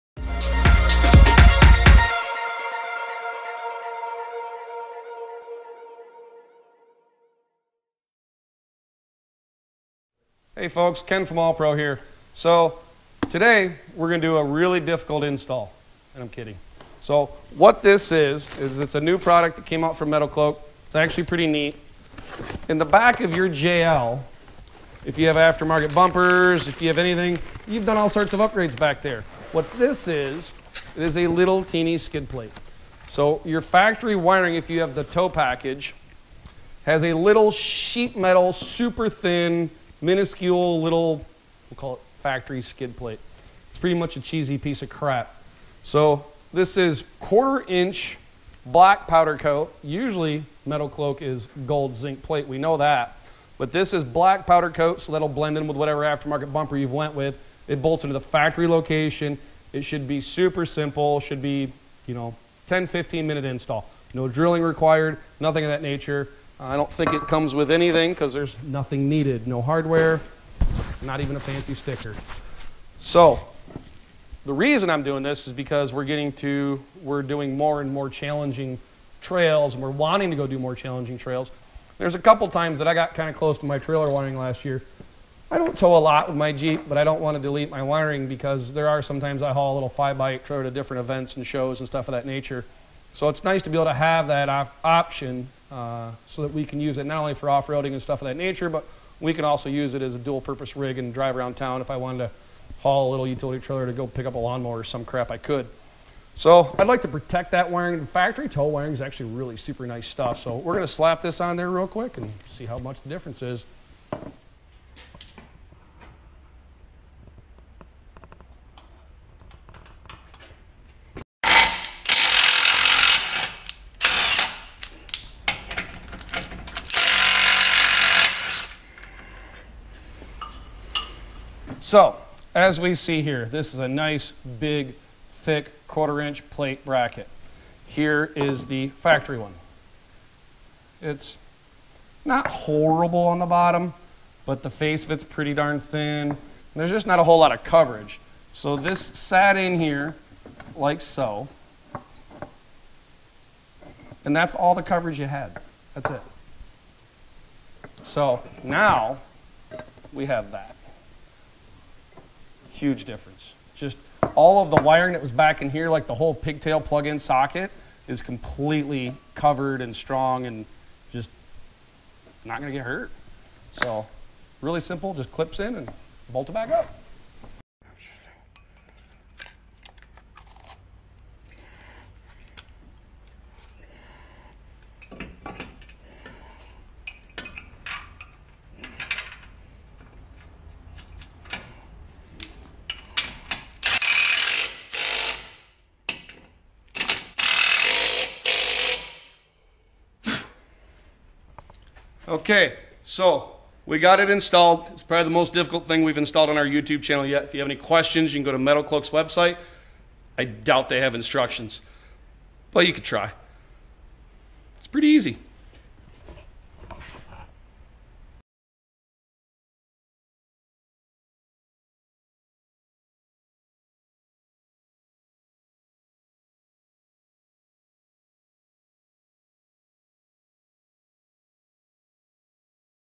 Ken Installs a Metalcloak Trailer Wiring Skid
This week we're installing a trailer wiring skid plate from Metalcloak.